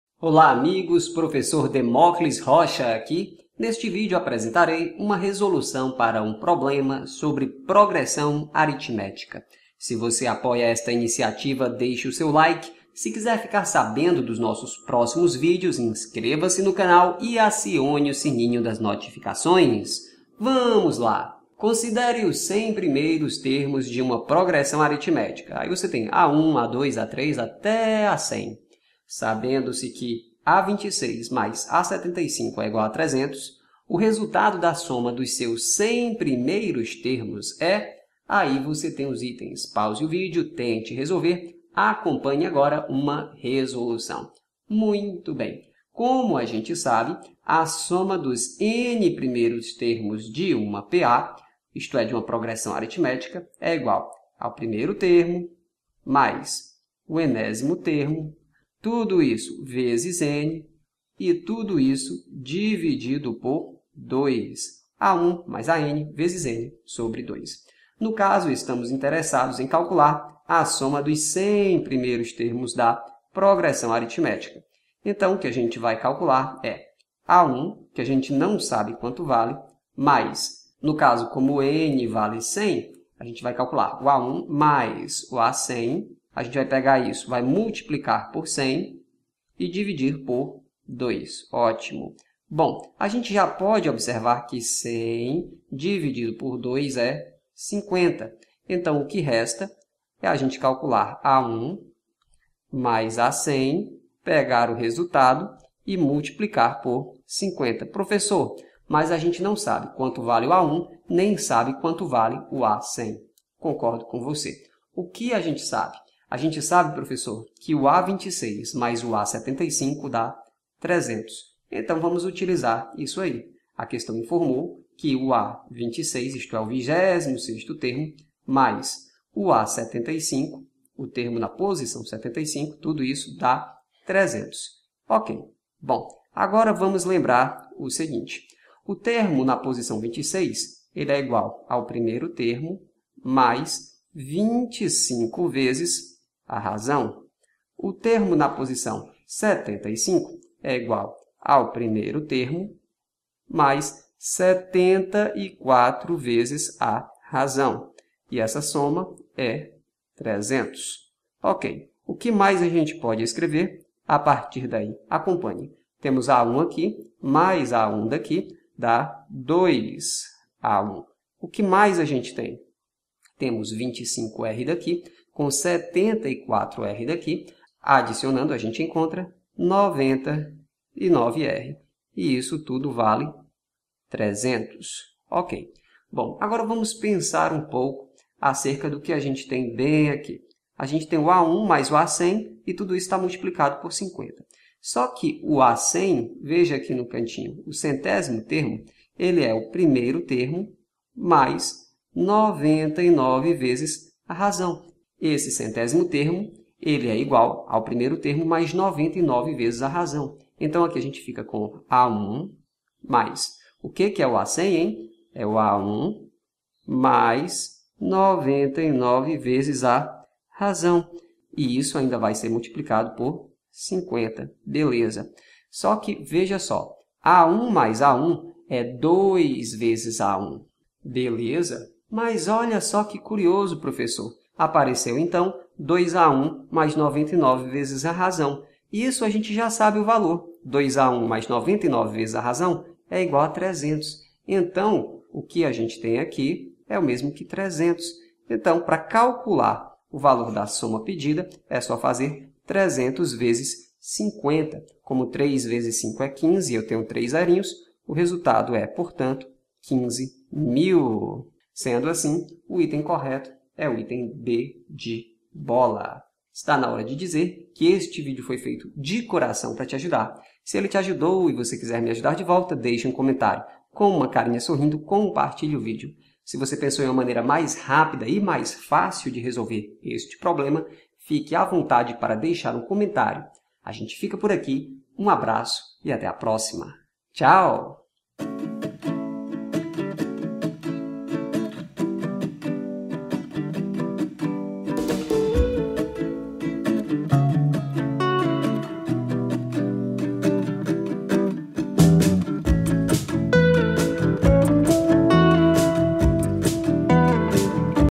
PROGRESSÃO ARITMÉTICA
Olá, amigos do meu canal no YouTube. Eu sou o professor Demóclis Rocha e, neste vídeo, apresentarei uma resolução para uma questão da Unesp sobre uma progressão aritmética.

(Unesp–SP) Considere os 100 primeiros termos de uma P.A.: {a_1, a_2, a_3, ..., a_100}. Saben-do-se que a_26 + a_75 = 300, o resultado da soma dos seus 100 primeiros termos é:
a)7650
b)15000
c)15300
d)30000
e)30300

CÂMERAS

MICROFONES